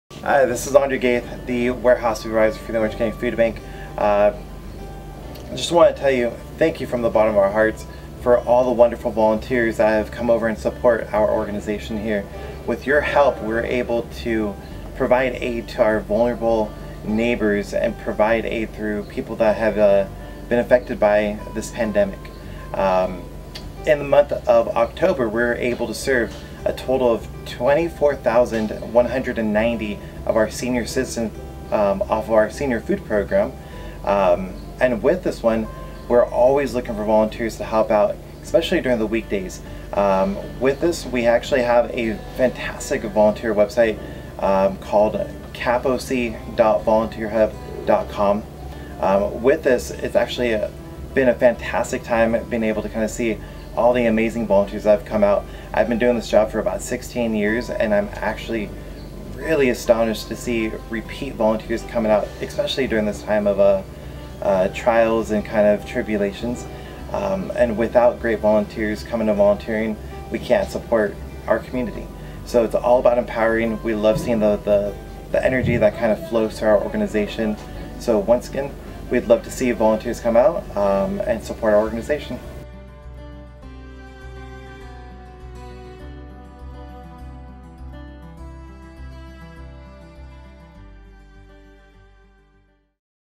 OC Food Bank Call for Volunteers - Nov 20
The OC Food Bank, located in Garden Grove, CA, is in need of volunteers now more than ever to help pack food boxes provided to vulnerable seniors, families, and individuals. Make an impact today and sign up a volunteer shift at capoc.volunteerhub.com